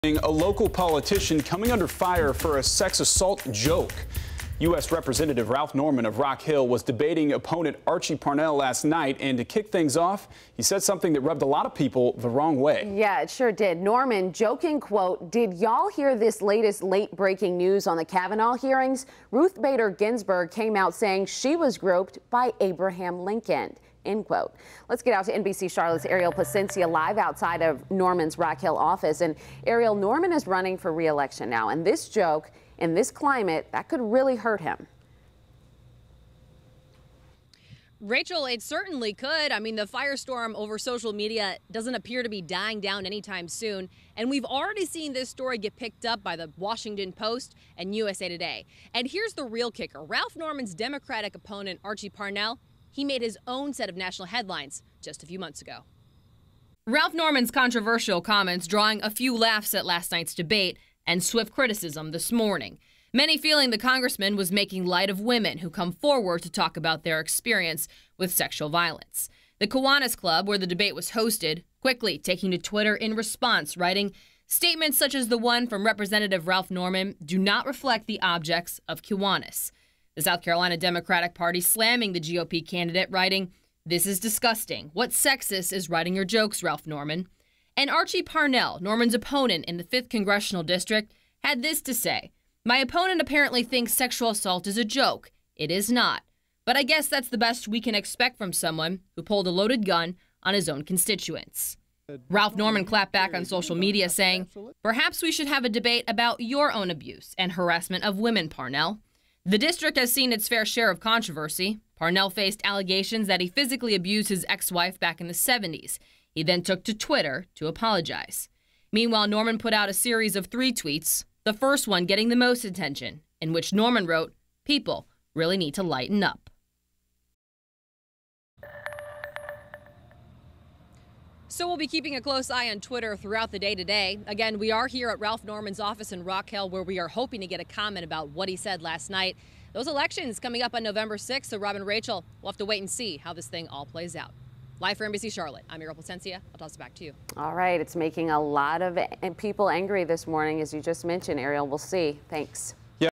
Ralph Norman makes sexual assault joke during debate
South Carolina Congressman Ralph Norman is making headlines after making a controversial joke about sexual assault and Supreme Court Justice Ruth Bader Ginsburg.